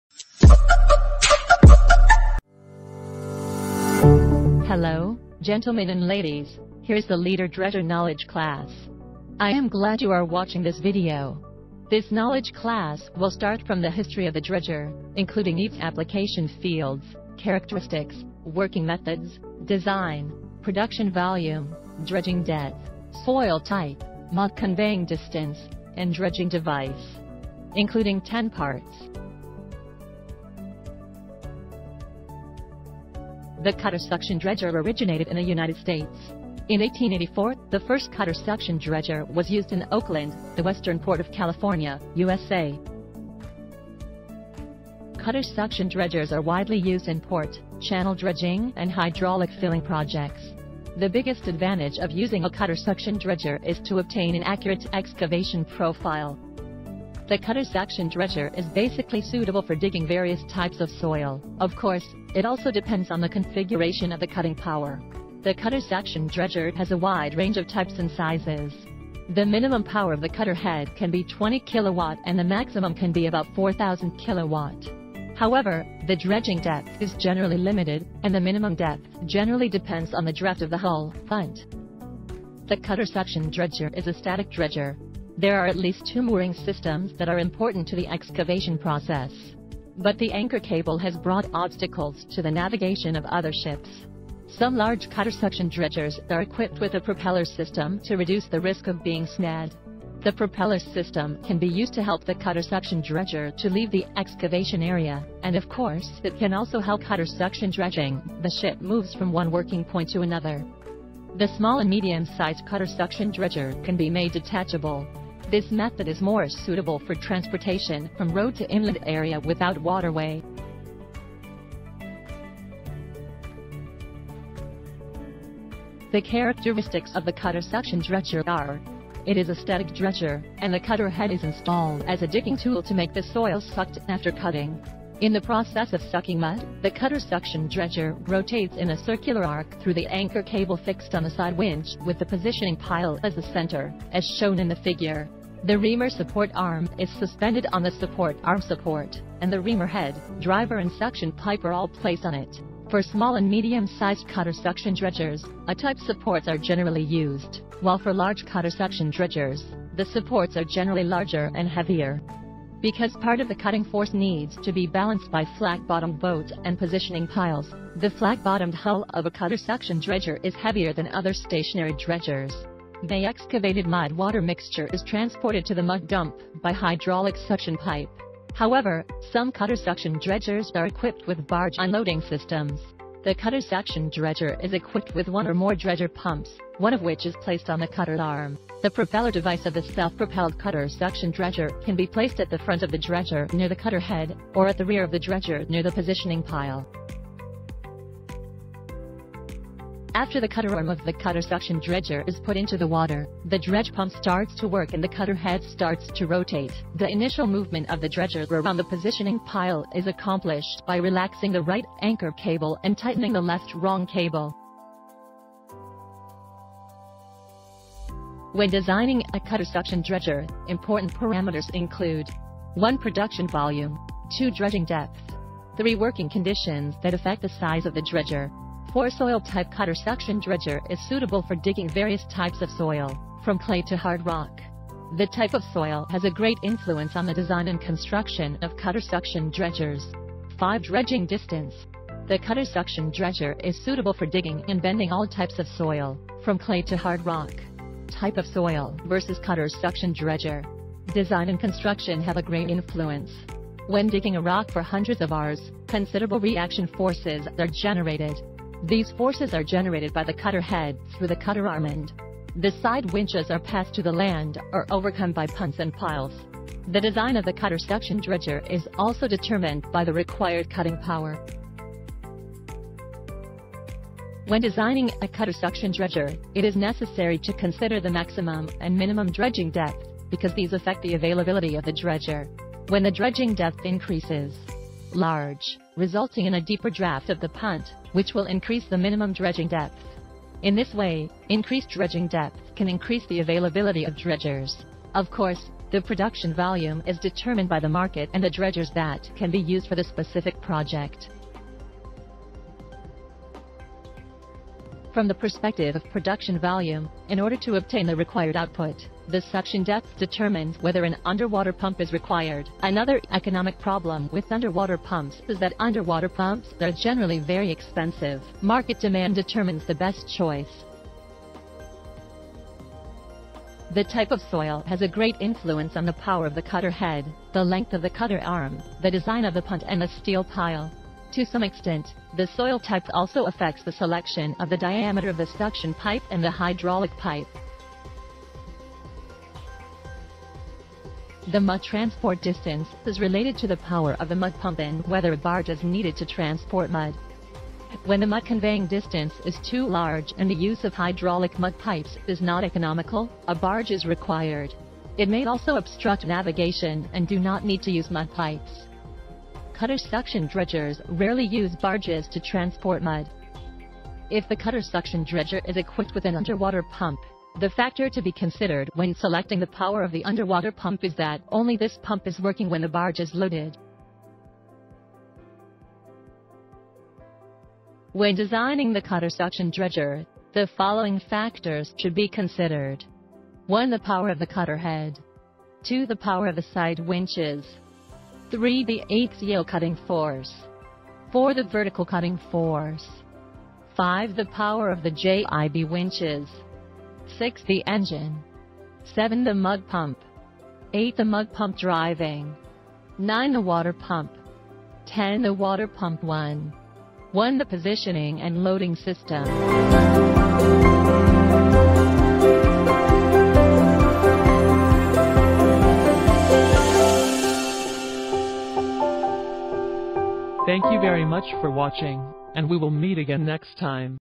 Dredging advice：basic highlights of cutter suction dredger for self-improvement(animotation)
[LeaderDredger_solution] Dredging knowledge from the leader dredger, as a dredger, you must have your own understanding and indirectness about cutter suction dredgers. This dredger animation video is compiled and produced by our designers to show you the cutter suction dredger.